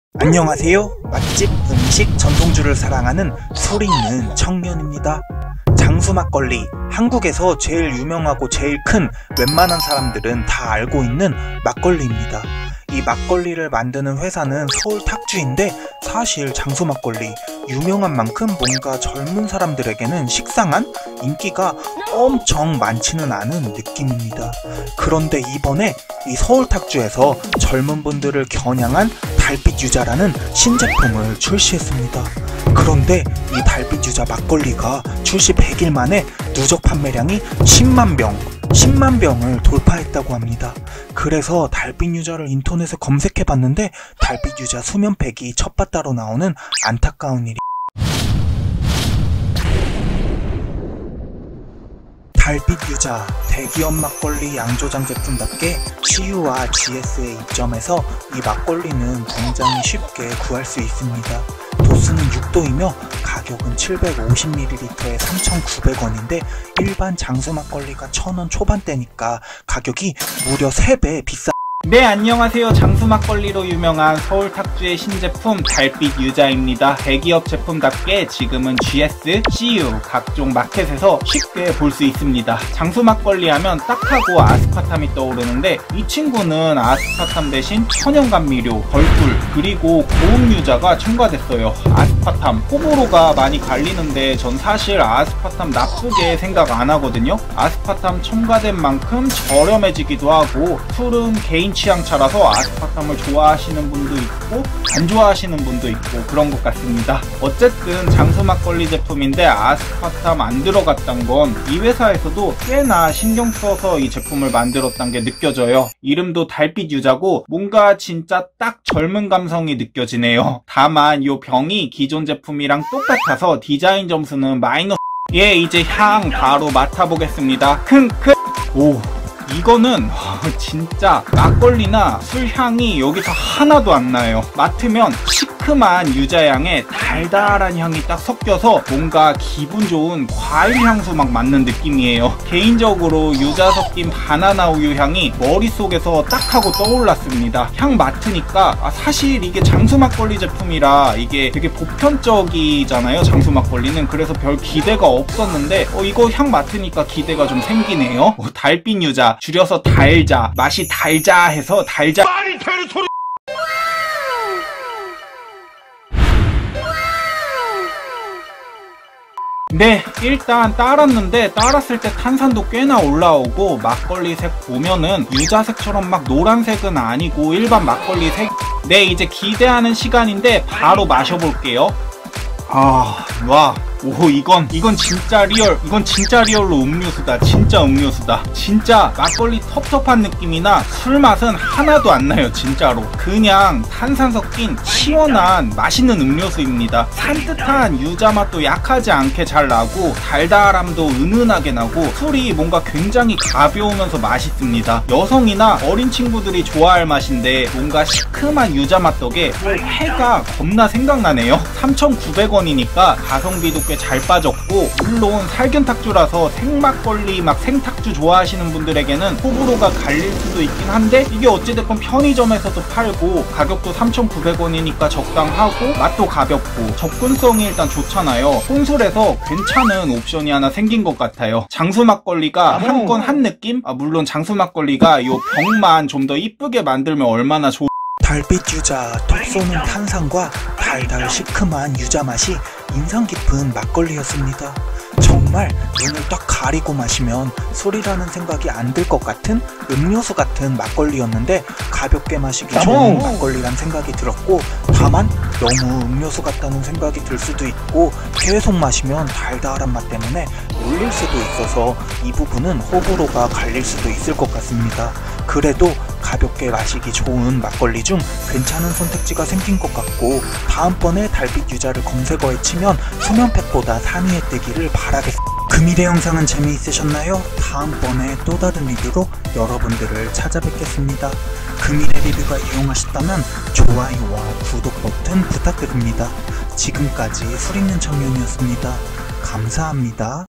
장수막걸리 신제품 달빛유자! 맛이 이렇다고!?; (막걸리 추천, 막걸리 리뷰, 혼술 추천, 혼술 리뷰, 알쓰 술 추천, 전통주 리뷰)_ Korean jangsu makgeolli
(There are English subtitles, too!)
본 리뷰는 주관적입니다.

안녕하세요~
장수 막걸리 신제품인 달빛유자를 마셔봤습니다.
이름에서부터 느껴지듯이
유자 맛 막걸리인데
이 막걸리의 맛은 과연 어떠할지
매우 주관적인 리뷰 시작하겠습니다.